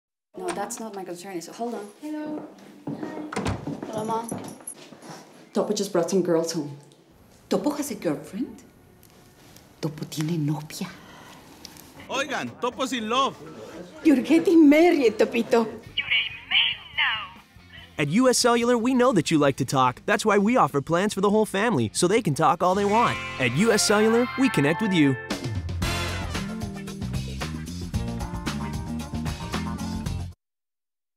Actress Sandra Nori in US Cellular Family Matters
Actress Sandra Nori opens this funny commercial about latin families in the US and their communication issues!